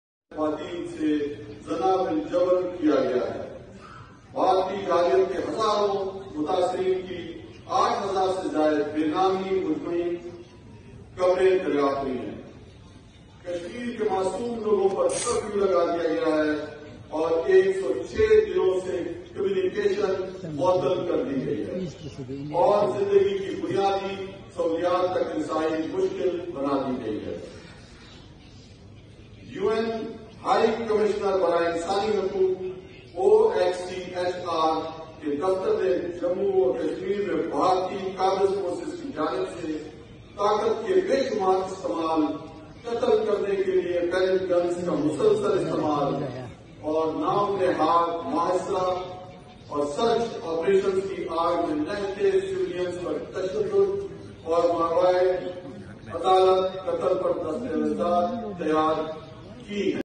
Today, world can witness RSS’s true face reflecting in Modi’s govt of so called secular democracy
‏بھارتی وفد کوبین الاقوامی سطح پر ایک بار پھر ذلت اور شرمندگی کا سامنا
کمبوڈیامیں ایشیائی پیسیفک سمٹ 2019 میں ڈپٹی اسپیکر قومی اسمبلی اور بھارتی وفد میں جھڑپ
ڈپٹی اسپیکر قاسم سوری مقبوضہ کشمیر میں بھارتی مظالم پر تقریر کررہے تھے
‏بھارتی وفد کی ڈپٹی اسپیکر قومی اسمبلی کی کشمیر پرتقریر روکنے کی کوششیں
بھارتی پارلیمنٹیرین وجے جولی کو سیکیورٹی اسٹاف نے ہال سے باہر نکال دیا
ڈپٹی اسپیکر قومی اسمبلی قاسم سوری نے شورشرابے کےباوجودتقریر کر ڈالی.